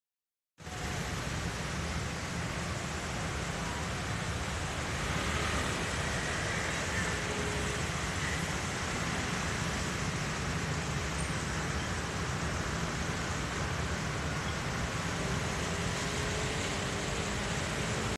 桃園 桃鶯路淹水 桃鶯路與樹仁三街路口 20190702 13
2019/7/2 暴雨轟炸北台灣,桃園市.桃園區-[桃鶯路]為桃園通往新北市鶯歌重要幹道,再次淹水,警方封閉多個路口,詳細影片與圖文如下:

2012/8/2 桃園市.桃園區-[桃鶯路]淹大水過往紀錄,詳細影片與圖文如下: